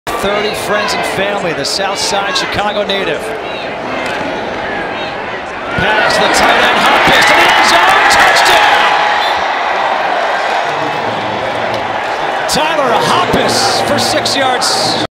Tyler Hoppes 6-Yard Touchdown vs. Illinois
Nebraska's Tanner Lee completes a pass to Tyler Hoppes for a 6-yard touchdown against Illinois in the second quarter.